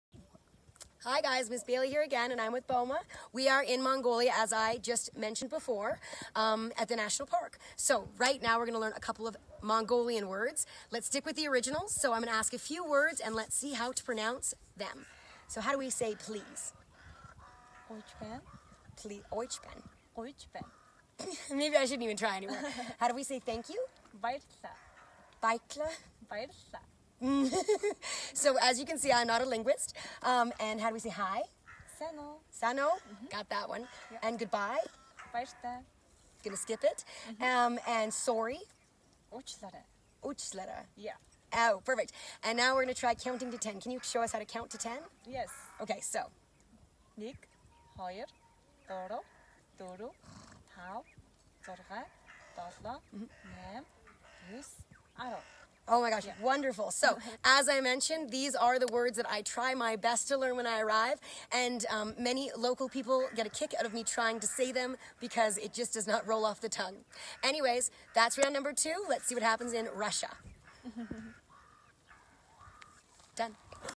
8 - Language Lesson in Mongolian
Thank you to my lovely tour guide for helping me learn a few words in Mongolian!